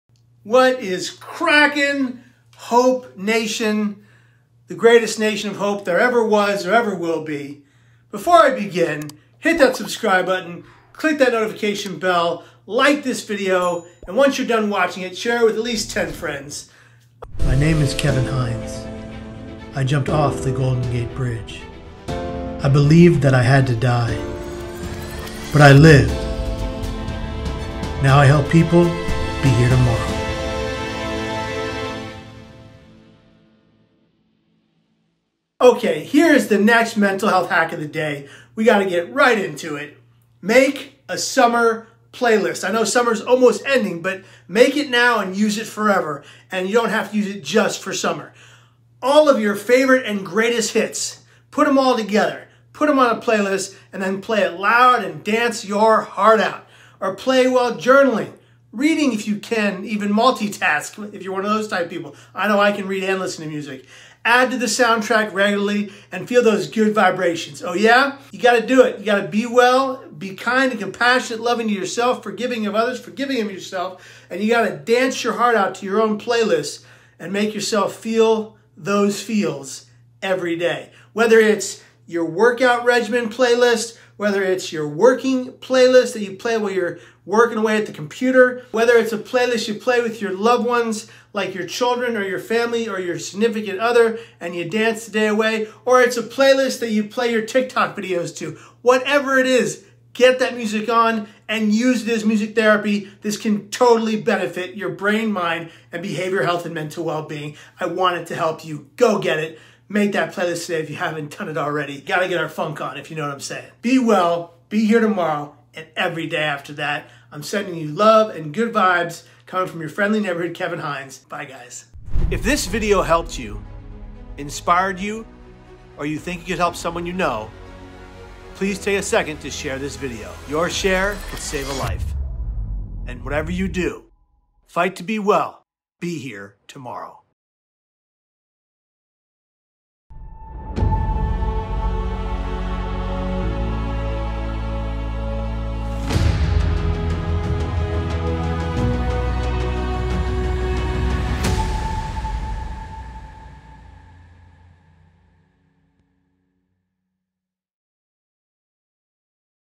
MAKE A SUMMER PLAYLIST: YES EVEN THOUGH ITS WINTER LOL
Music makes the body move, make your body and mind feel all kinds of good with a great music playlist. Music can change your brainwaves shift your mood. Put a wellness music playlist together today, don't wait another second.